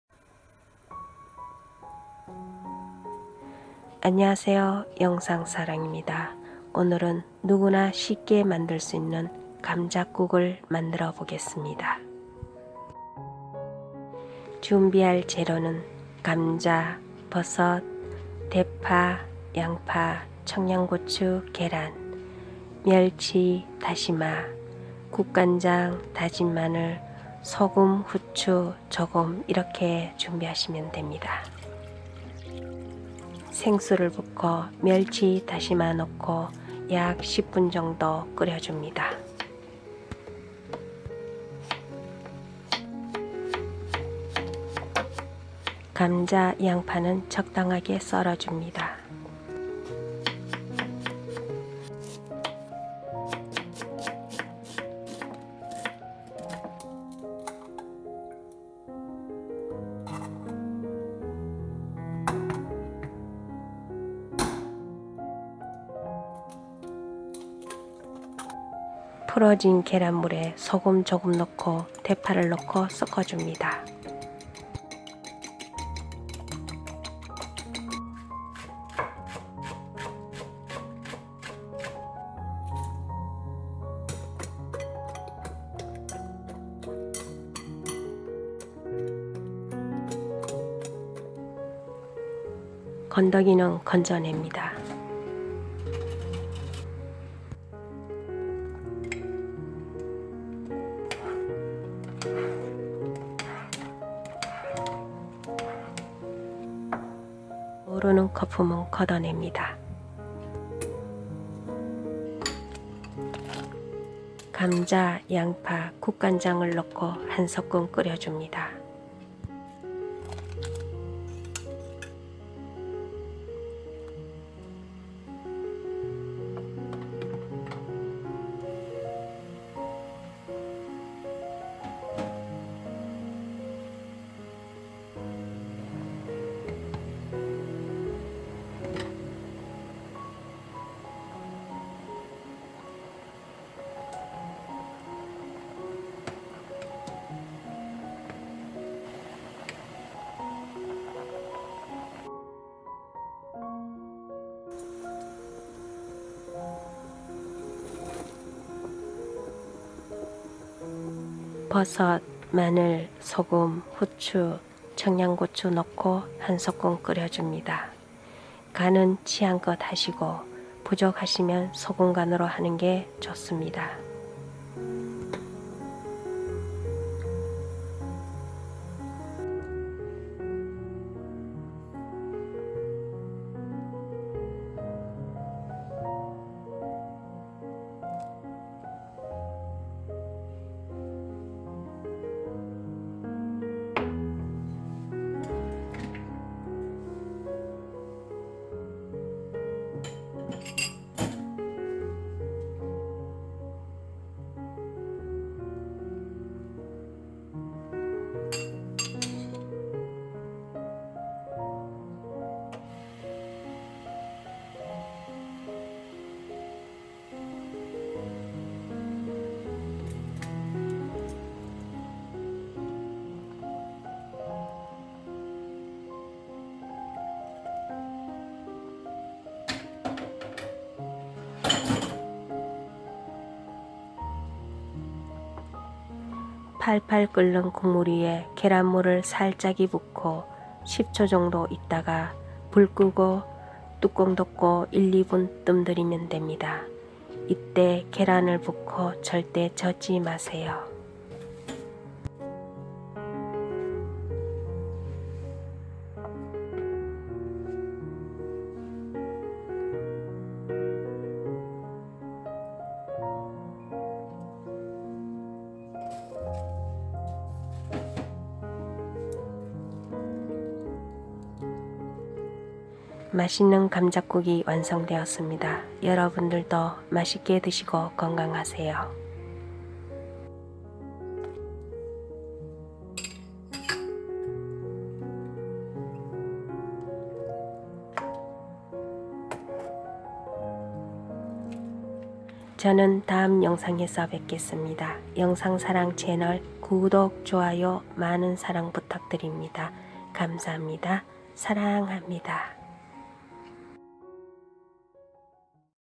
감자국 맛있게 끓이기 감자버섯국 맛있게 끓이는법
감자국 만들기

재료

감자  버섯 대파 양파 청양고추 계란

멸치 다시마 (국물 만들기)

국간장   다진마늘  소금 후추 조금